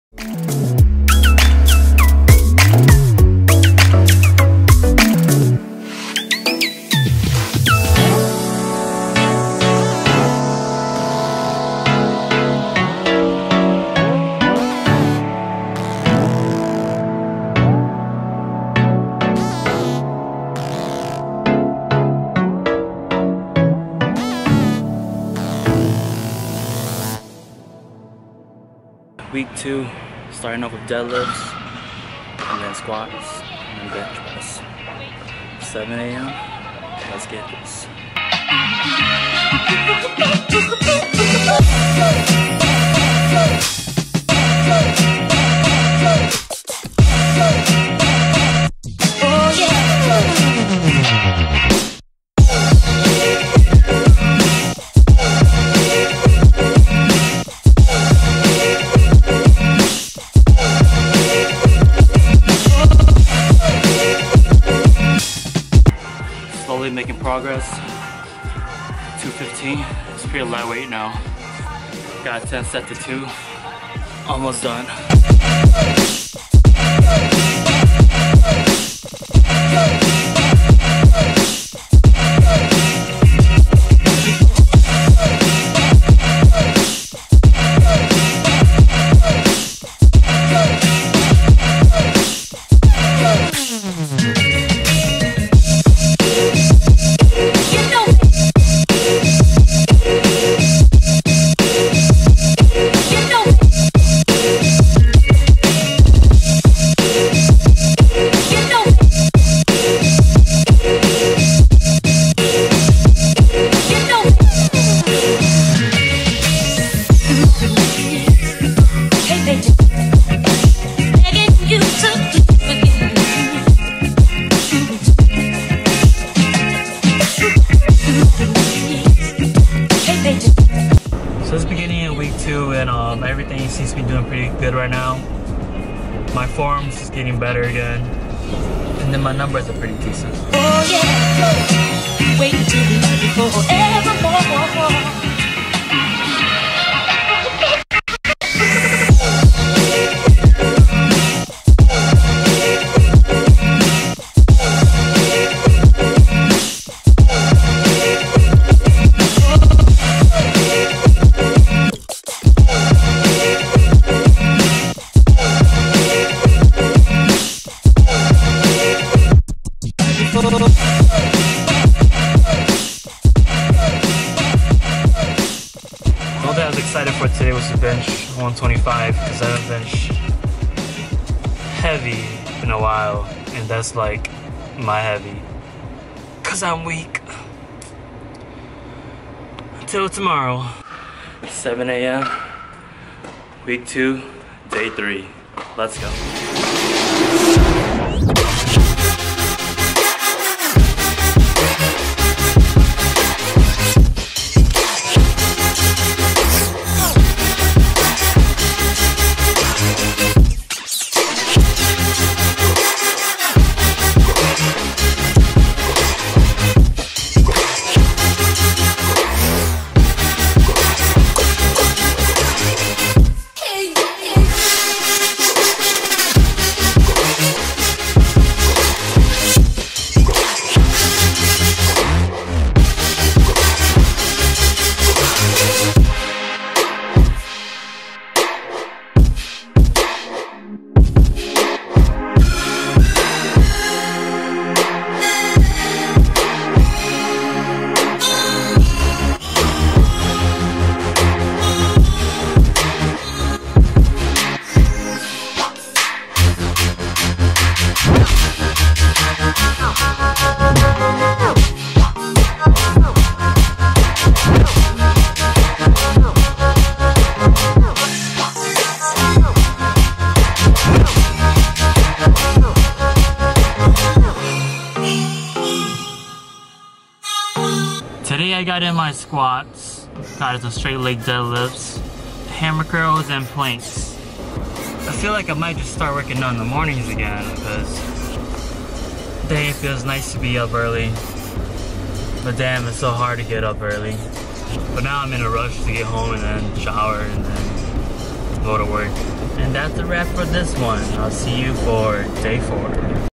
Week 2 | Current Physique
Week 2 is done! Feeling strong and want to PR or lift heavier than what my program is having me doing, but I need to trust in the program... Can't let the ego get to me.
- 2/12 Weeks Done
- Possible PL Competition in June